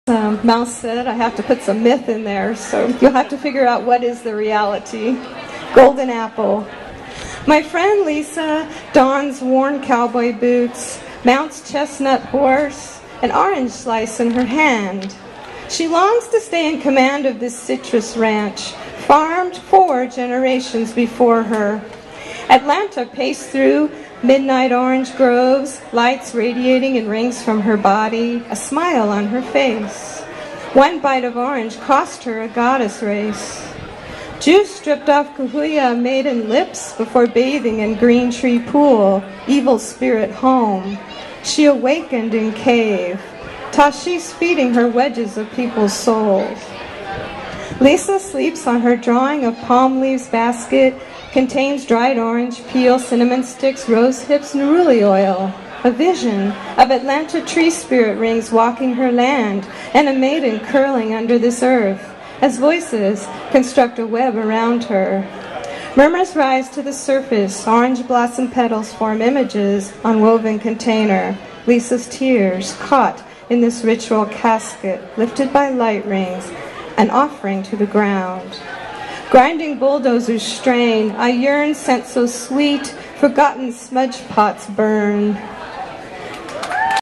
Cindy Rinne- "Golden Apple" live from Art in the Alley 6
Poet Cindy Rinne reads her poem "Golden Apple" at Art in the Alley in Redlands, Ca.